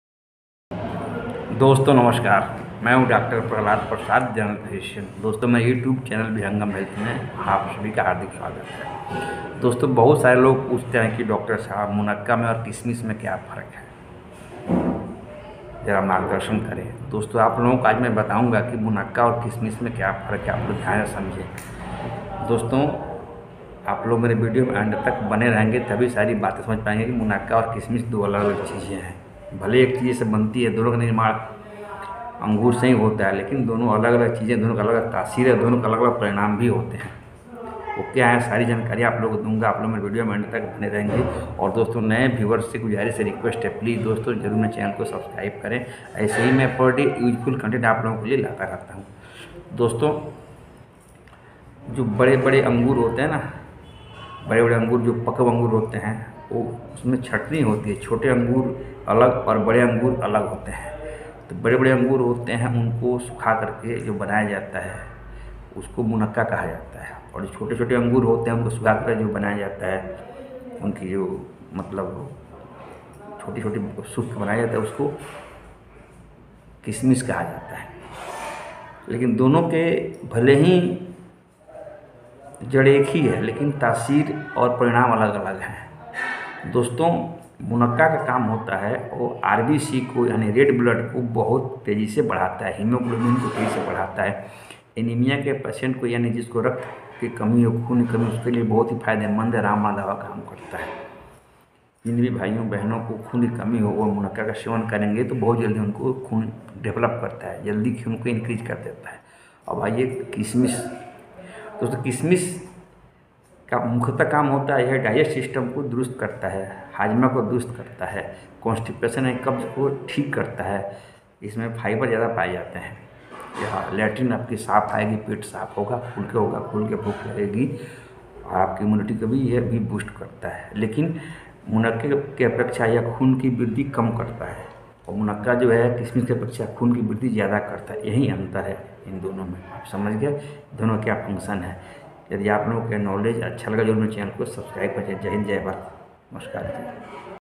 किशमिश और मुनक्का में क्या फर्क हैdifference between kishmish and munakka#Vihangam health.
What is the difference between kishmish and munakka.
Health benefits of kishmish.
Health benefits of munakka.
Health tips.
Vihangam health#hindi.